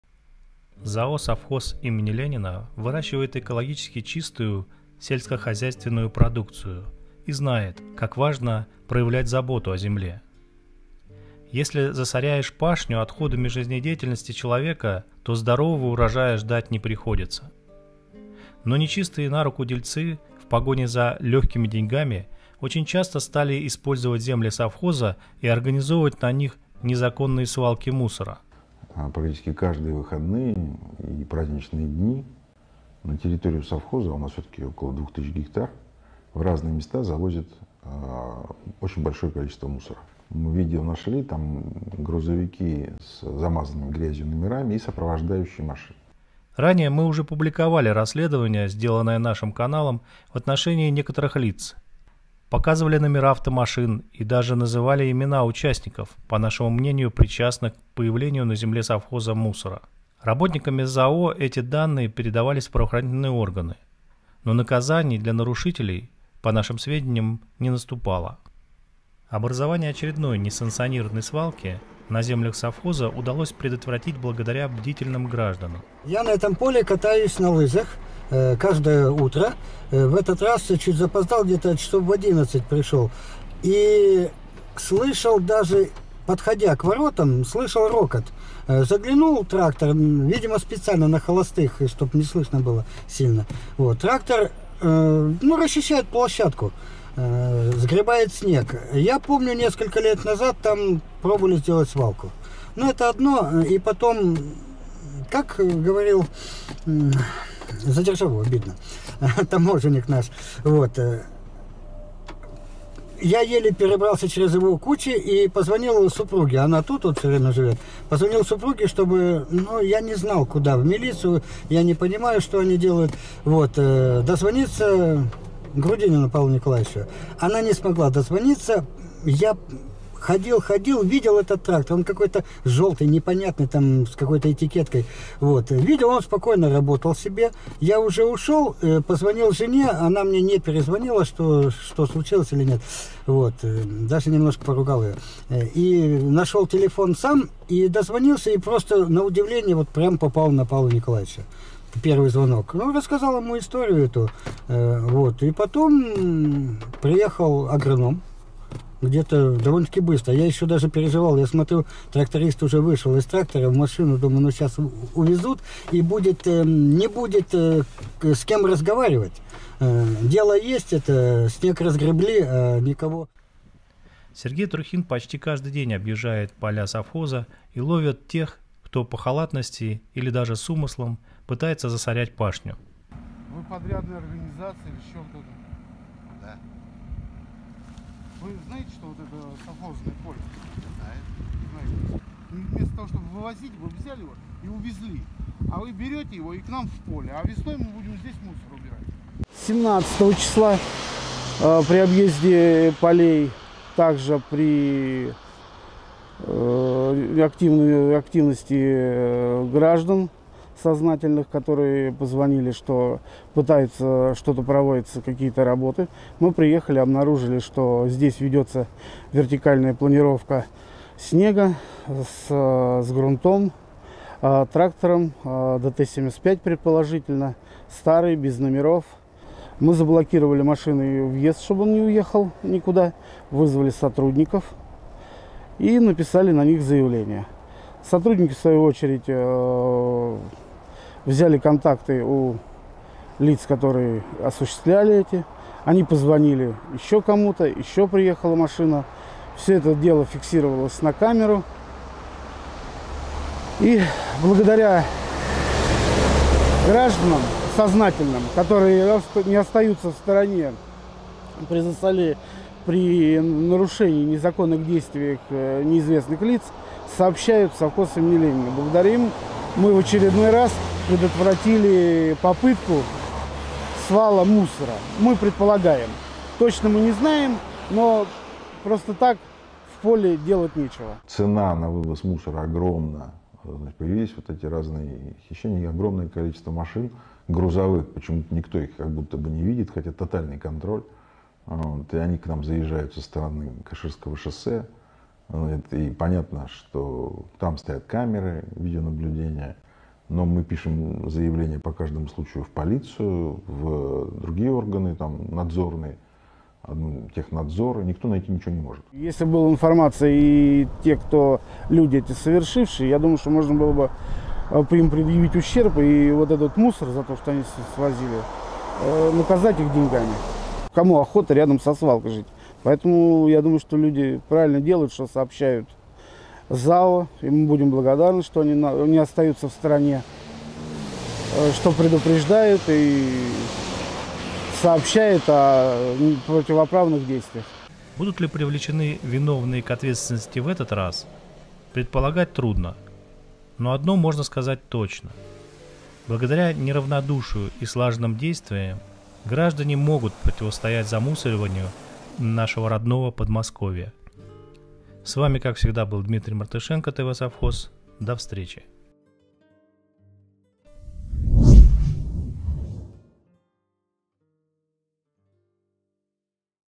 Бдительный гражданин предотвратил возможно самый массовый вывоз мусора на поля совхоза
ЗАО "Совхоз имени Ленина" выращивает экологически чистую сельскохозяйственную продукцию и знает, как важно проявлять заботу о земле. Если засорять пашню отходами жизнедеятельности человека, то здорового урожая ждать не приходиться.

Но не чистые на руку дельцы, в погоне за легкими деньгами, очень часто стали использовать земли совхоза и организовывать на них не законные свалки мусора.

Ранее мы уже публиковали расследование сделанное нашим каналом в отношении некоторых лиц, показывали номера автомашин и даже называли имена участников, по нашему мнению, причастных к появлению на земле Совхоза мусора.

Работниками ЗАО эти данные перековались в правоохранительные органы, но наказаний для нарушителей, по нашим сведениям, не наступало.

Образование очередной не санкционированной свалке на землях совхоза удалось предотвратить благодаря бдительным гражданам.

Сергей Трухин почти каждый день объезжает поля Совхоза и ловит тех, кто по халатности или даже с умыслом пытается засорять пашню.

Будут ли привлечены виновные к ответственности в этот раз предполагать трудно, но одно можно сказать точно. Благодаря не равнодушию и слаженным действиям, граждане могут противостоять замусориванию нашего родного Подмосковья.

С вами как всегда был Дмитрий Мартышенко ТВ Совхоз. До встречи.